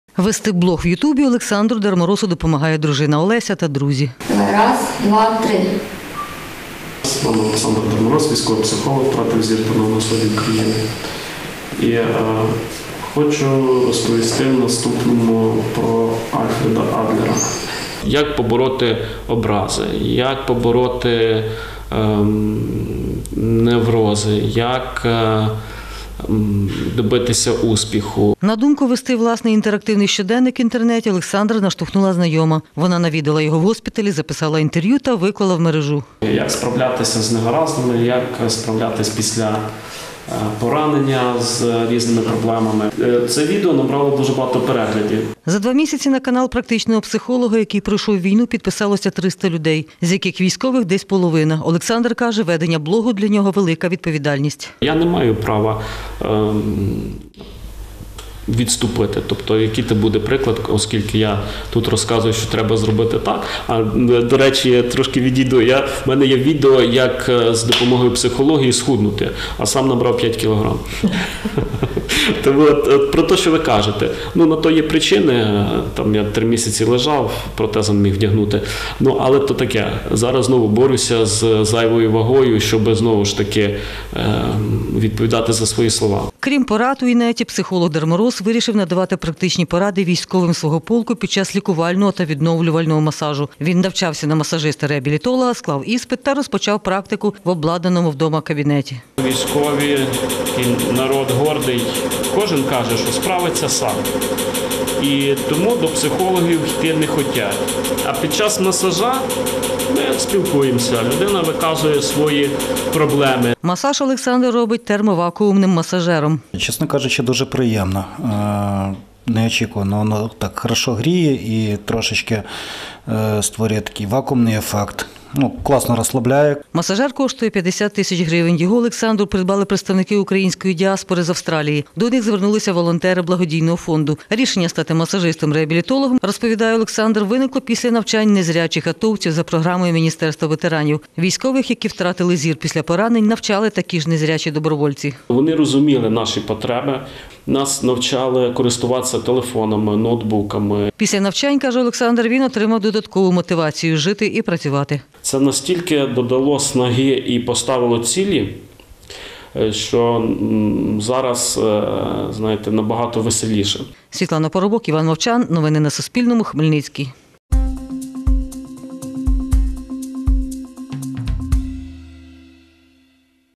Блог у соціальних мережах веде незрячий військовий психолог Олександр Дарморос
Особистий блог у соціальних мережах розпочав вести військовий психолог Хмельницького 8-го полку спеціального призначення Олександр ДарморОс. У березні 16-го року його поранили на сході України. Чоловік повністю втратив зір і ногу, переніс численні операції, лікувався та пройшов реабілітацію.